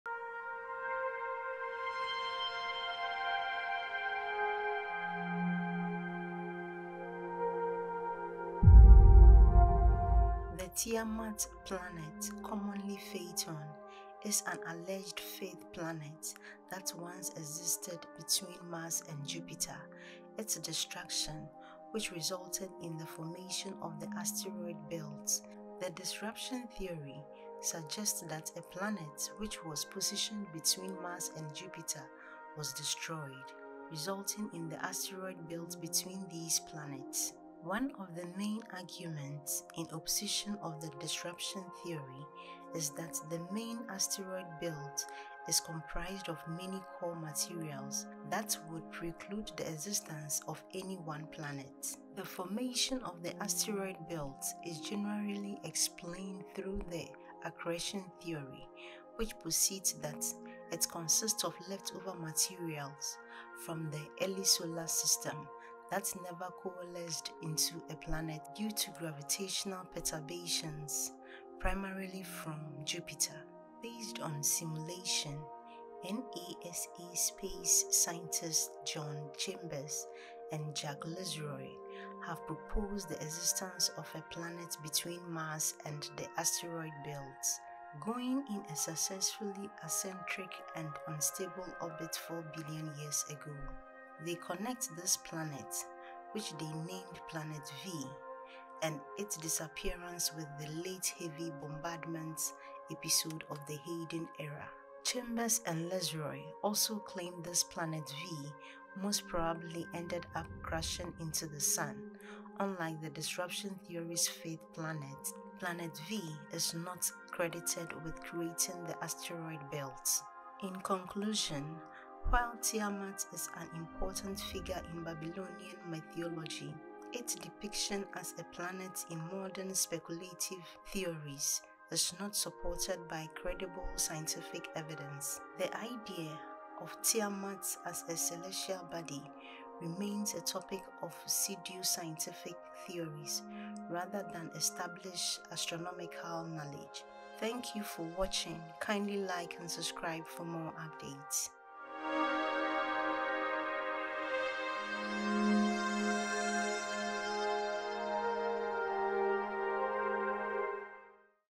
The Planet Tiamat
Tiamat is a hypothetical planet proposed in ancient Mesopotamian mythology, believed to have existed in the early solar system.

It was thought to have been destroyed in a cosmic collision, with its remnants forming the asteroid belt.

This idea is not supported by modern science but remains a fascinating part of mythological and speculative discussions about planetary formation.